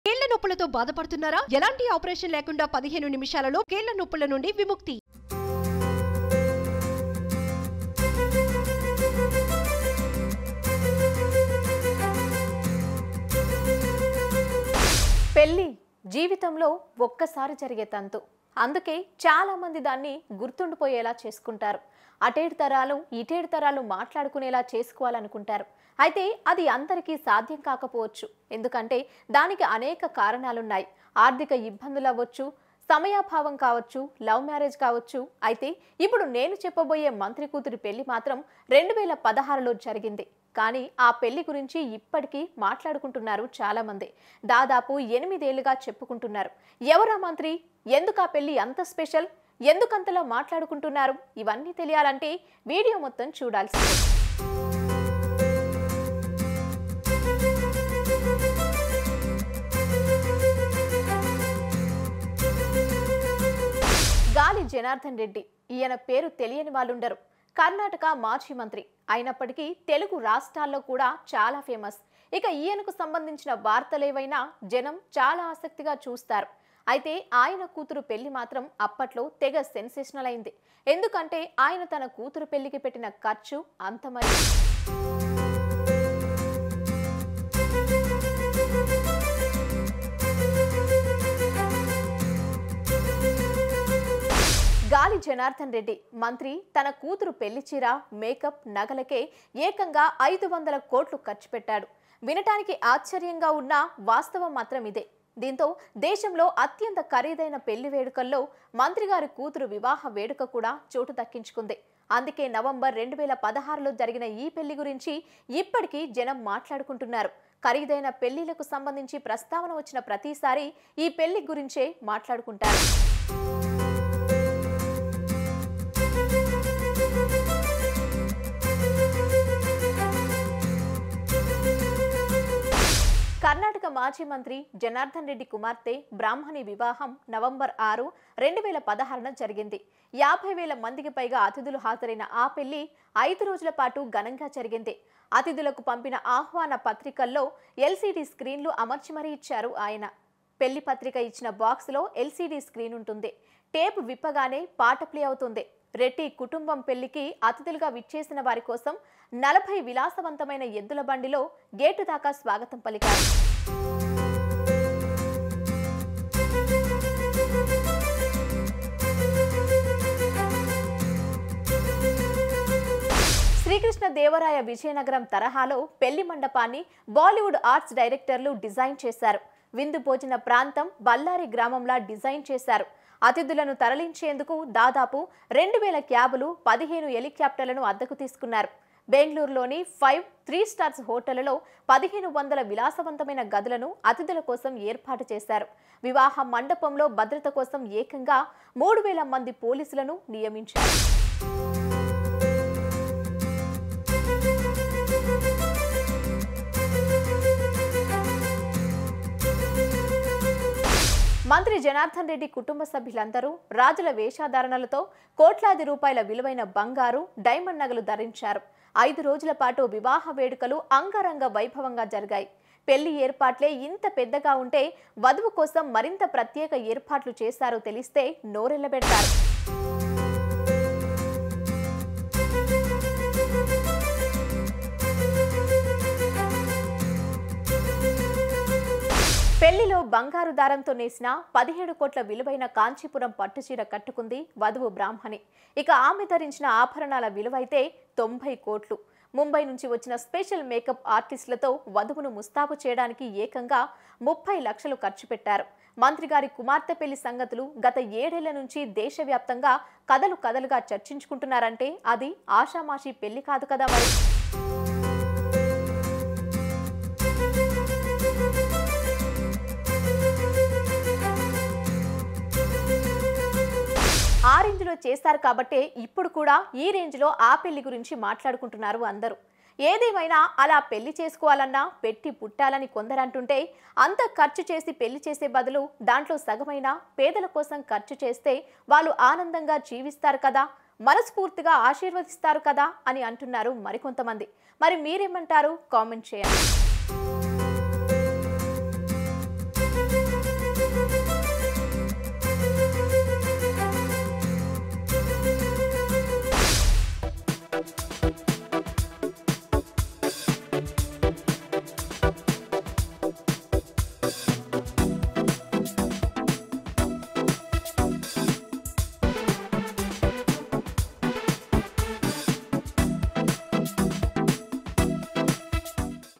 Gali Janardhan Reddy Daughter Wedding Saree Cost | 17 Crores Saree | 90 Crores Jewellery
Watch► Gali Janardhan Reddy Daughter Wedding Saree Cost | 17 Crores Saree | 90 Crores Jewellery